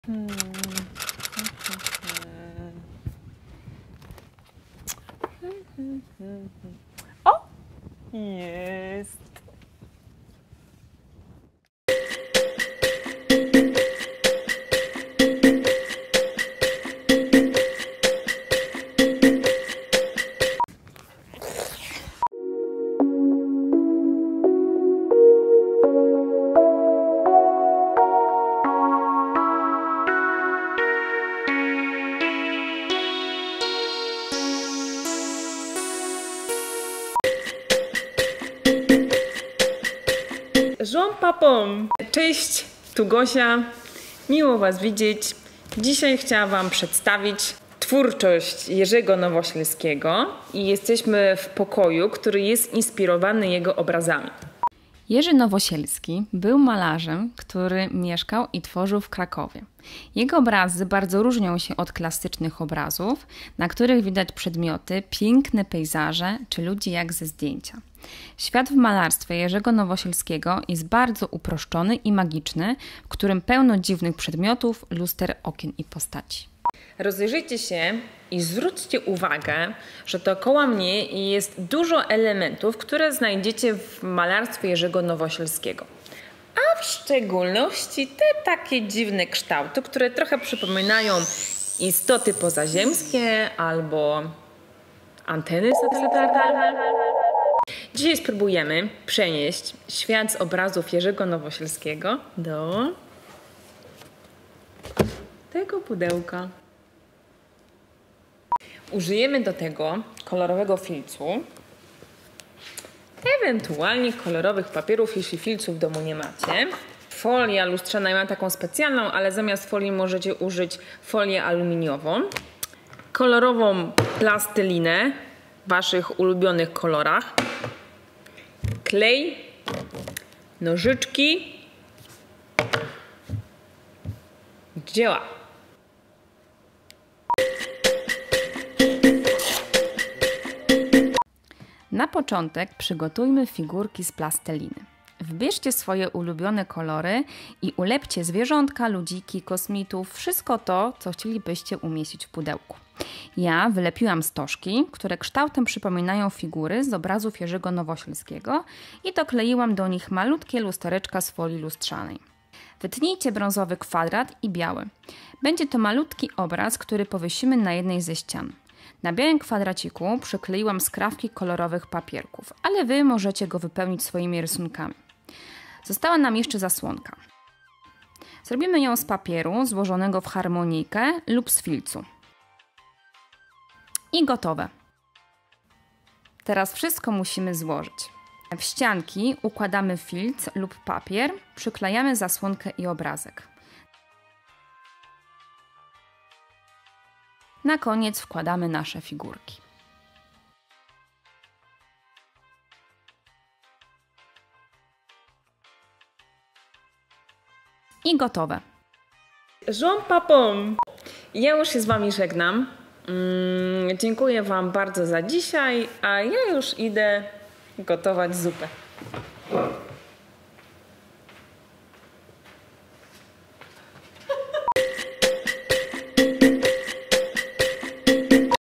Artystyczne Zajęcia Domowe z Gosią odc. 3 - Jerzy Nowosielski
Trzeci odcinek programu wprowadzi Was w świat magicznych obrazów Jerzego Nowosielskiego. Miłej zabawy!
Prowadzenie: Małgorzata Jagielska
Realizacja video: Piotr Czyż